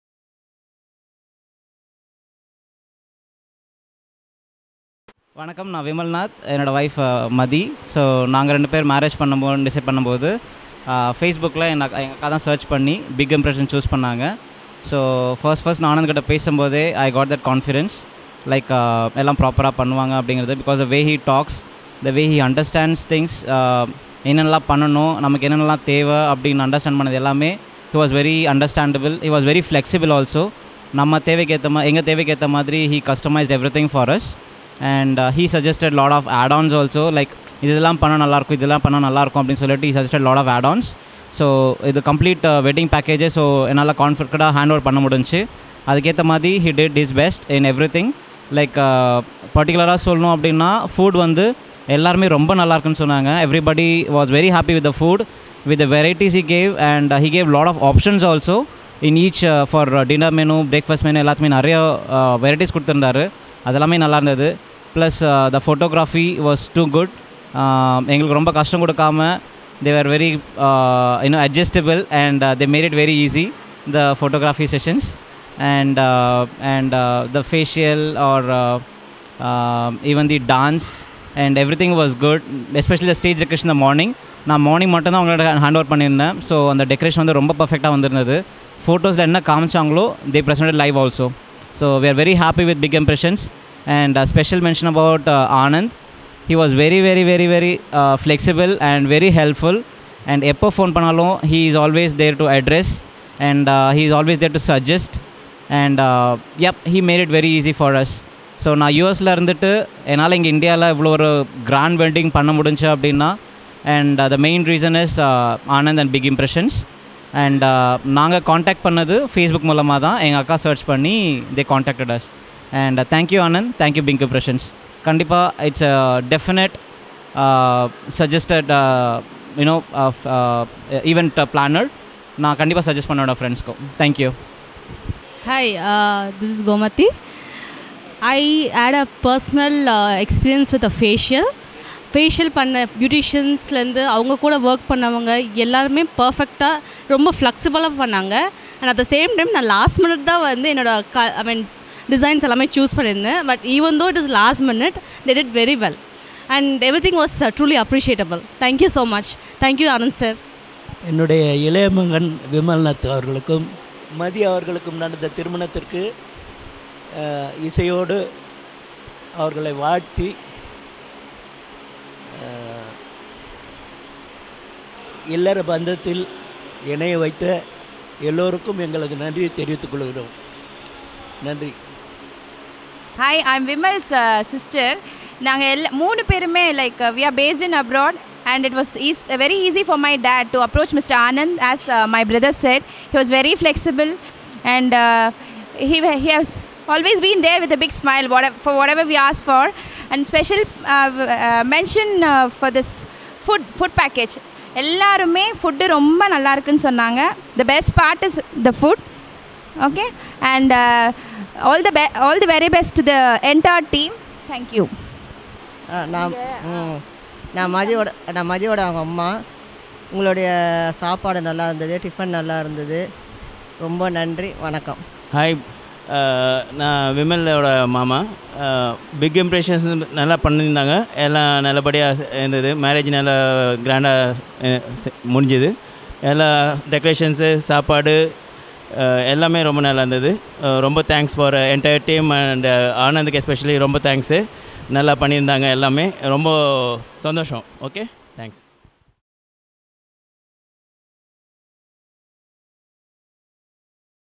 Customer Review for our Complete Wedding Package APM Mahal Gudiyatham
Satisfied Customer shares their Experience with Big Impressions Events.